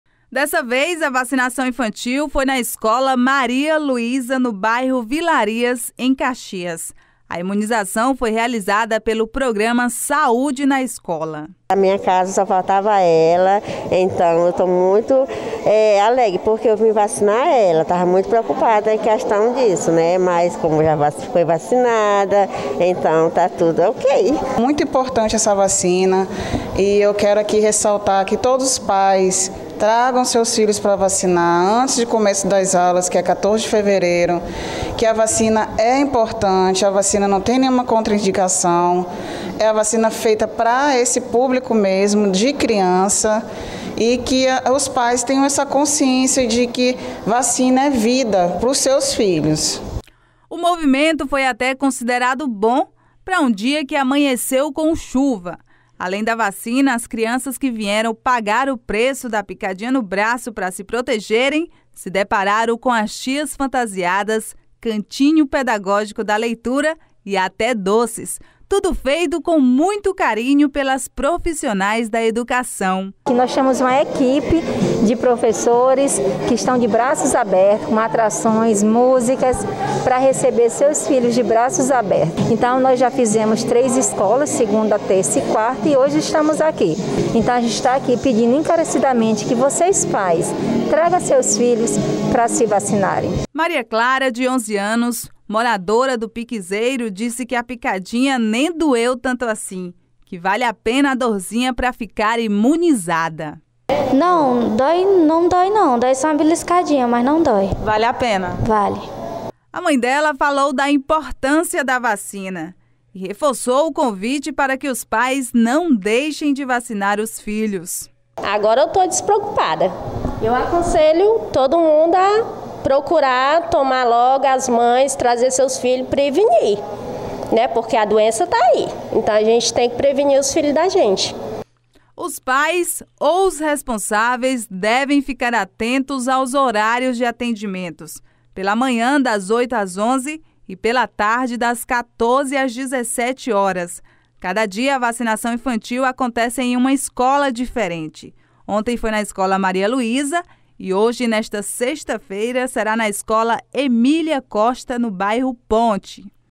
PSE MANTÉM VACINAÇÃO INFANTIL CONTRA COVID-19 NAS ESCOLAS MUNICIPAIS
Acompanhe-nos pelo Instagram Facebook Confira também os nossos programas 📻No rádio, sintonize: 105,9 Confira nossa programação completa